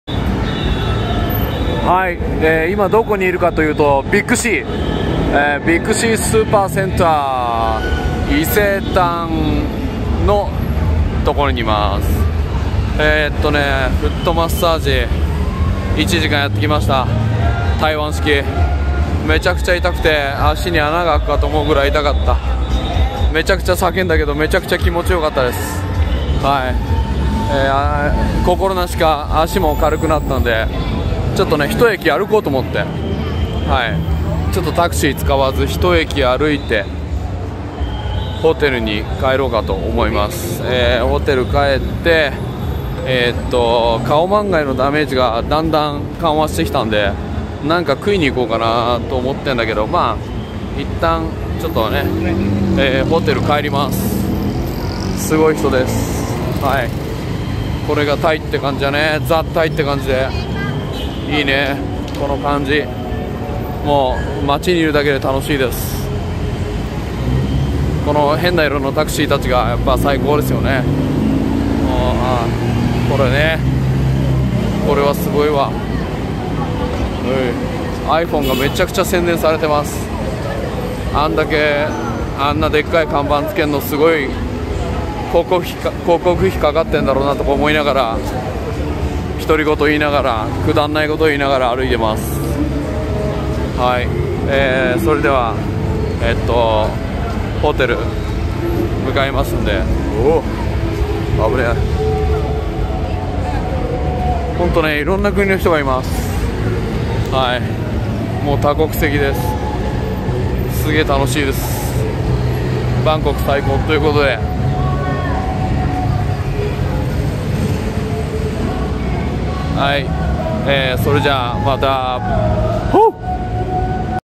フットマッサージ終わっての感想を散歩しながら。 バンコク旅行
チットロム駅近くのミスターフィートでマッサージ終わっての感想を散歩しながら☺️